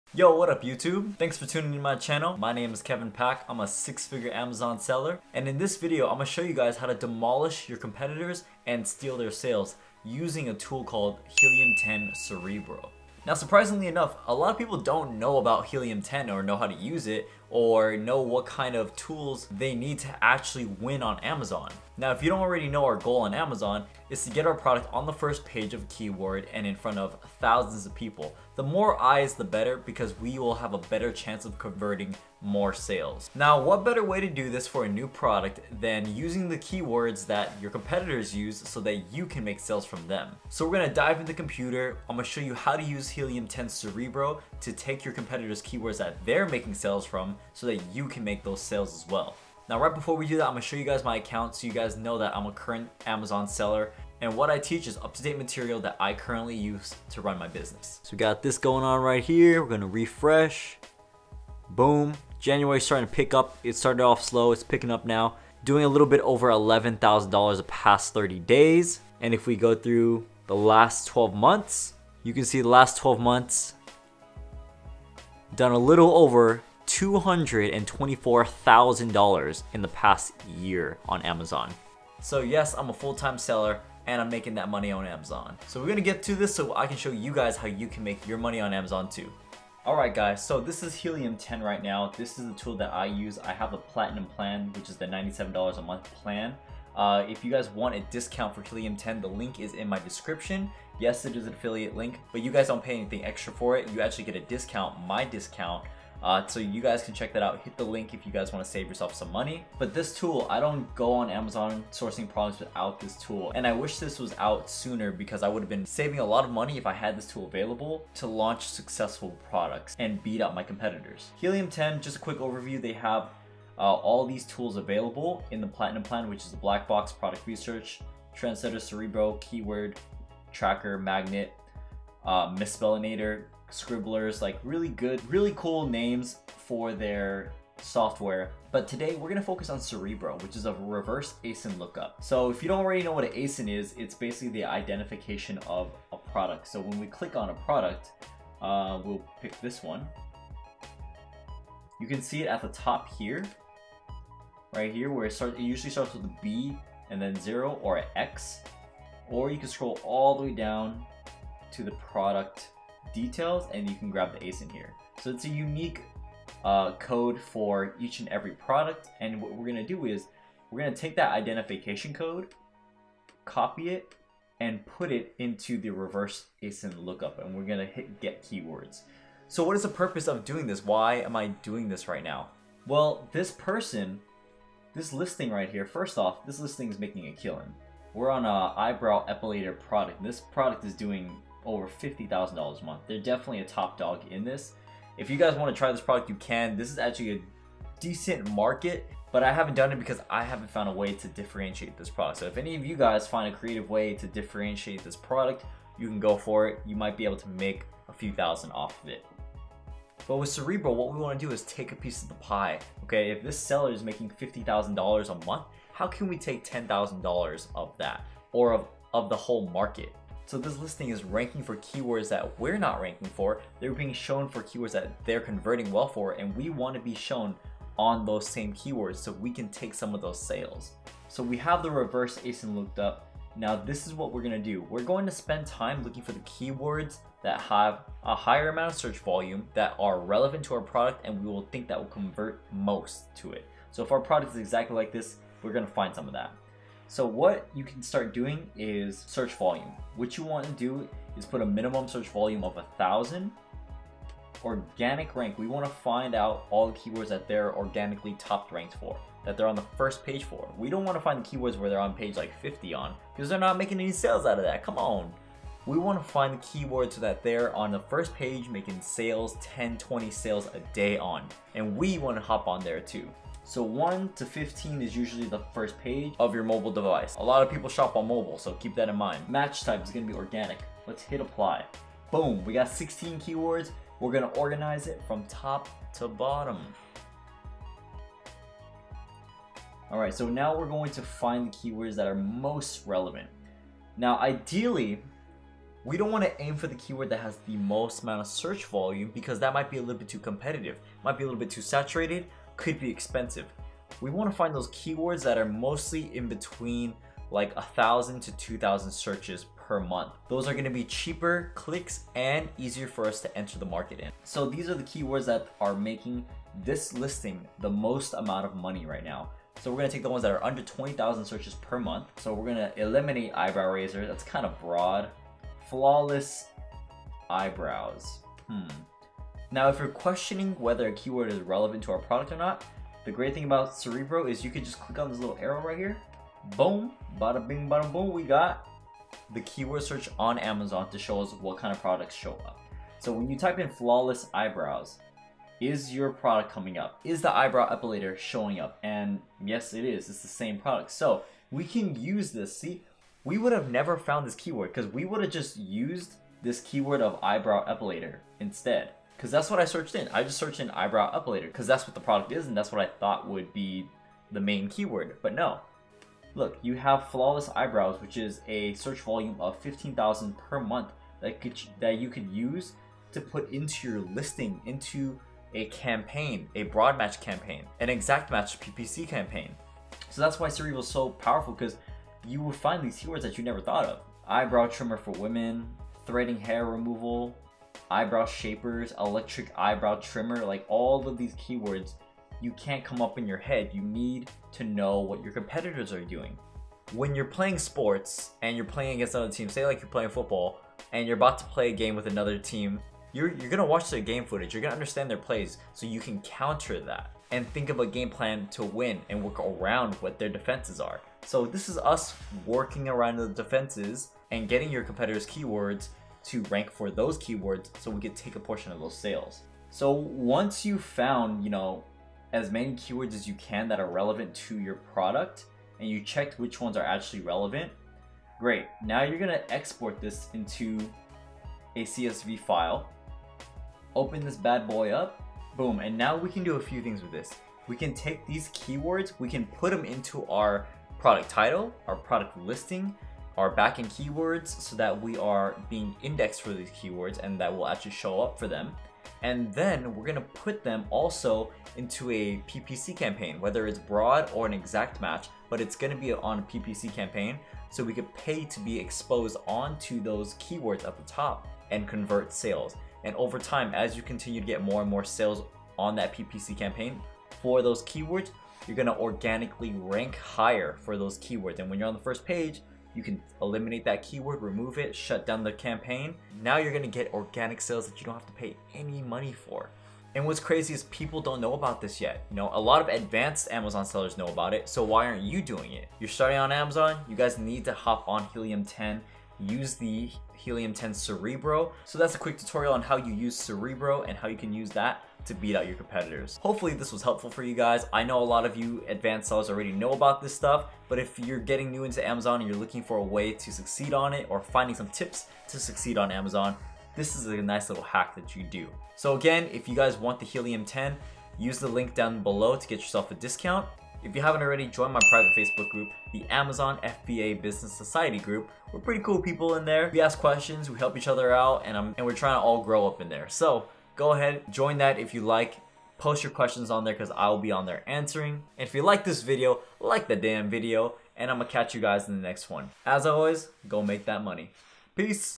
DEMOLISH Your Competitors & INCREASE Your Sales Using HELIUM10 CEREBRO! AMAZON FBA 2020 STEP BY STEP
Helium10 is great tool for amazon sellers to increase their sales and grow their business! One of my favorite tools is helium10 cerebro which is  a reverse asin tool that lets you see what keywords your competitors rank and make sales on! We are gonna steal that and increase our sales and win!